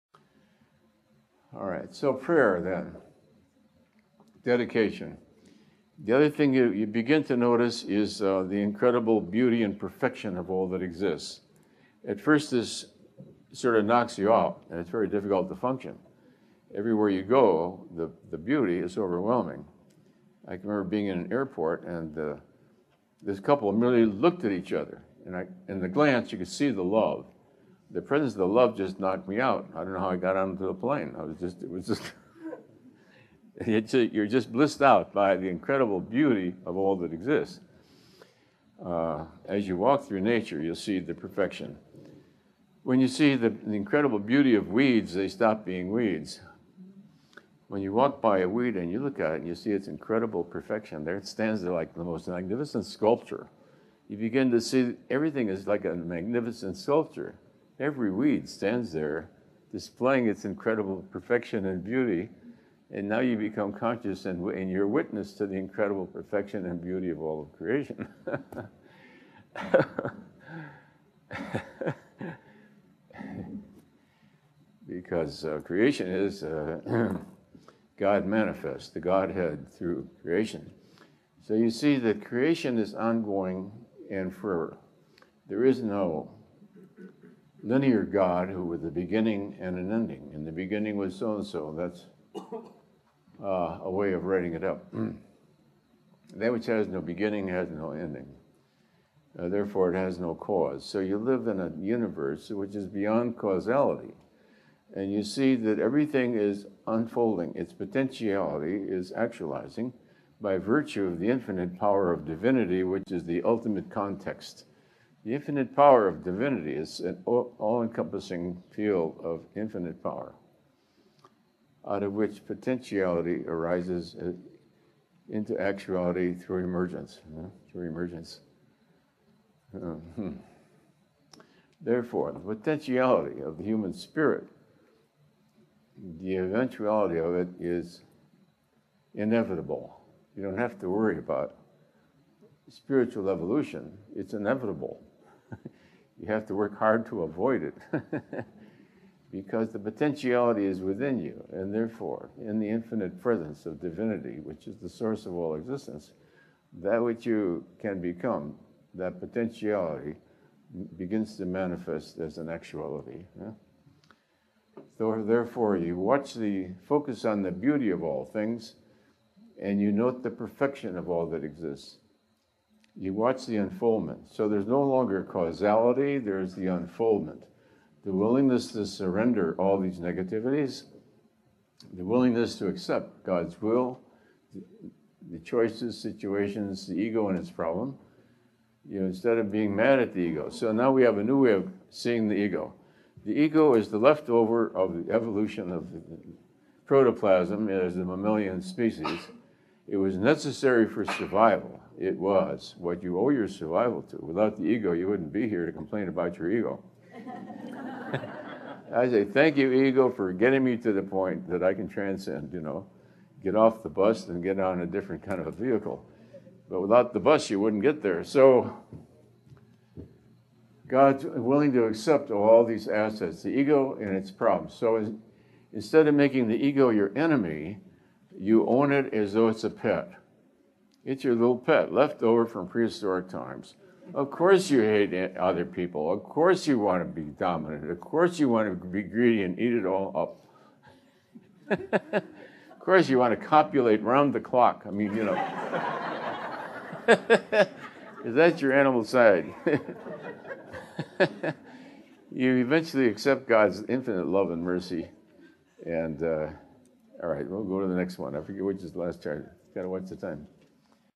Volume VI How to Raise Your Level of Consciousness
Spiritual evolution is inevitable, since the potential to evolve in consciousness is there within us.
Find out more with this loving and informational clip.

Full lecture is available through Audible.